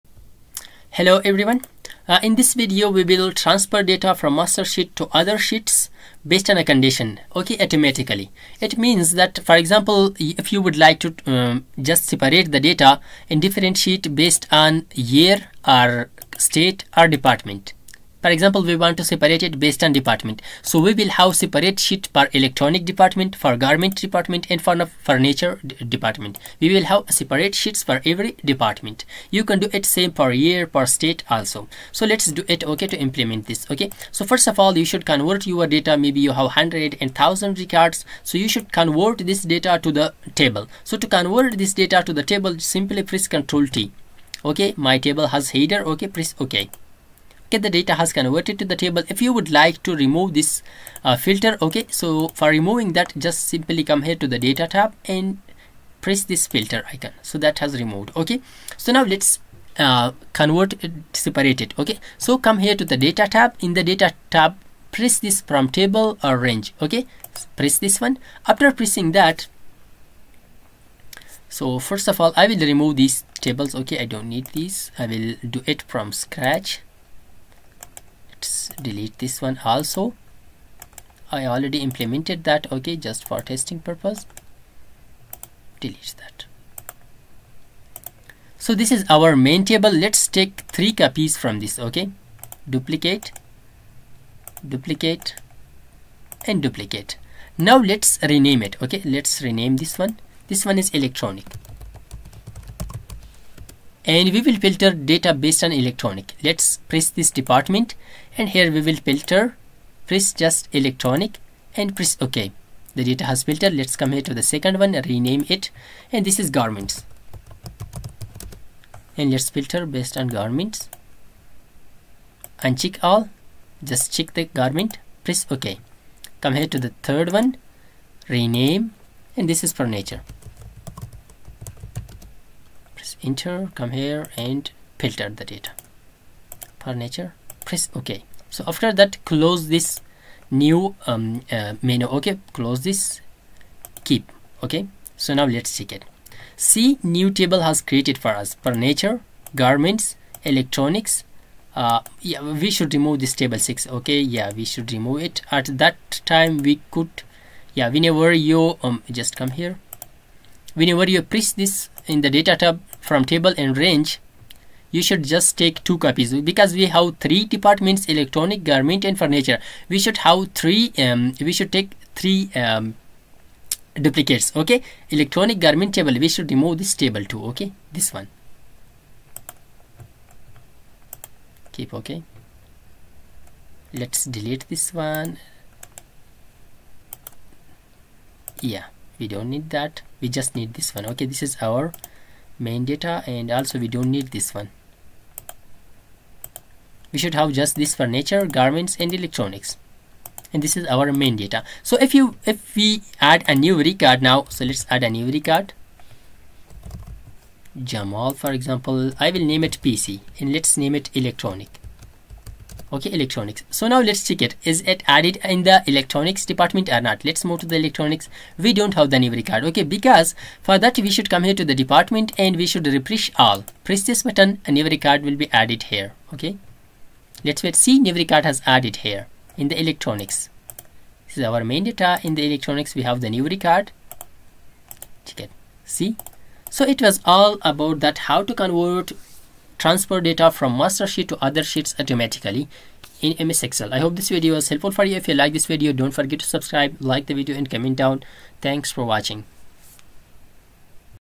Excel Hack: Say Goodbye to Copy-Pasting! Automate Data Transfer Across Sheets
Tired of tedious copy-pasting in Excel?  Unleash the power of automation and transfer data seamlessly between sheets!  Learn pro techniques to organize your data effortlessly and save hours of time.  In this video, you'll discover:

**Hidden Excel gems to automate data transfer **
**Step-by-step methods for every skill level **
**Tips to keep your data organized and updated **
**Unlock a whole new level of Excel mastery! **

Ready to transform your Excel workflow?  Ditch the manual labor and create dynamic, self-updating spreadsheets!  This video unveils the secrets of automatic data transfer, empowering you to:

**Break free from repetitive tasks **
Master multiple methods for different scenarios ️
**Keep your data organized and efficient **
Impress your colleagues with your Excel wizardry ✨